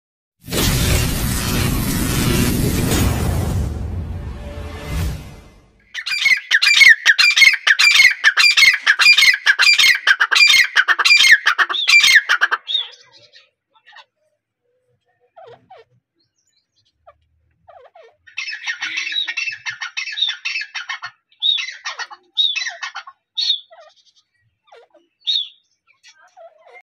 Dhakni and irani Teetar ki Awaaz , Female Teetar Sound -Teetar Bulane Ki Awaz
Female Teetar Sound - Jungli Teetar Bulane Ki Awaaz
dhakni teetar ki awaz
pair of dhakni teetar
desi teetar ke awaz